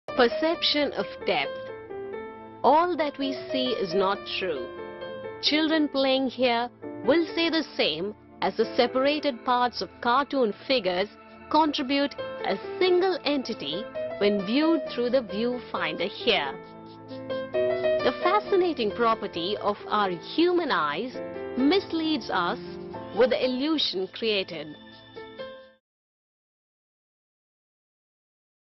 Perception of depth.mp4
What we see all is not true is simply explained with these fascinating figures. The property of our retina is explained with this gadget.